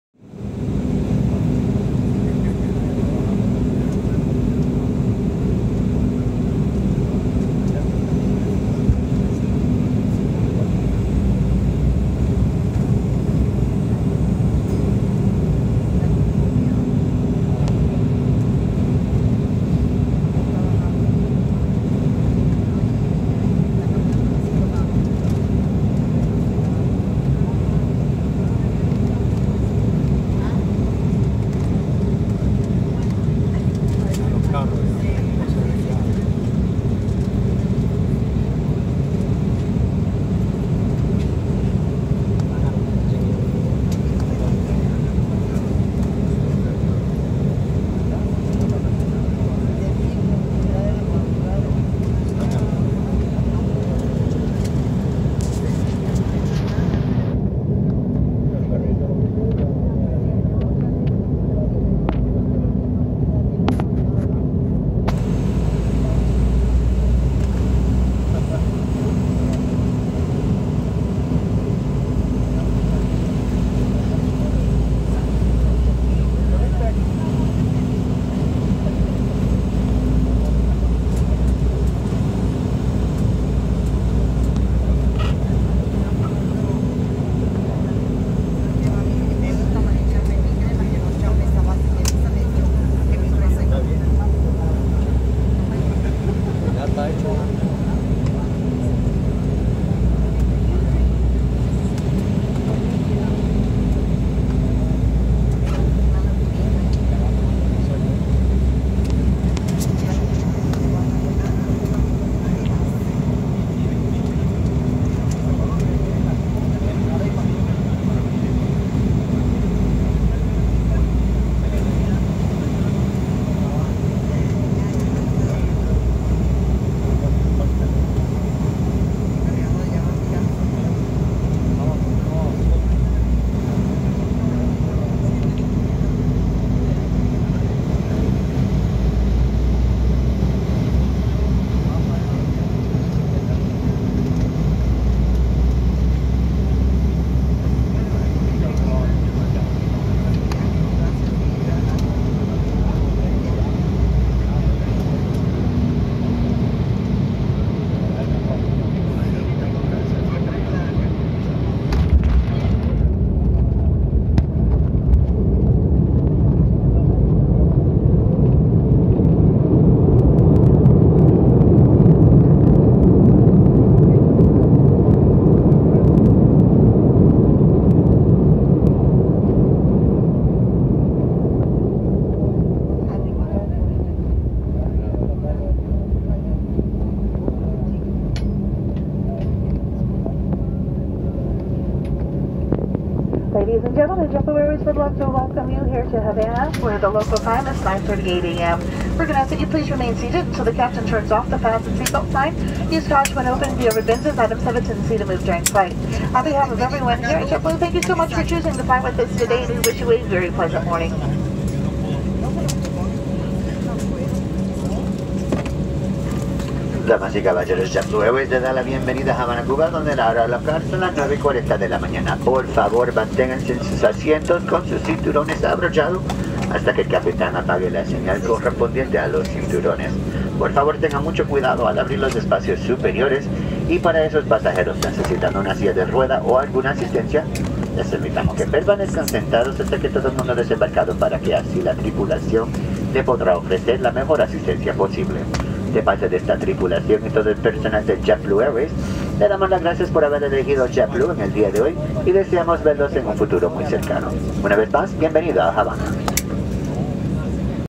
Jet Blue Landing in Havana
JetBlue Flight from Orlando to Havana.  Kudos to JetBlue for both their flights from Orlando and to Ft. Lauderdale.  Professional crew and great flights.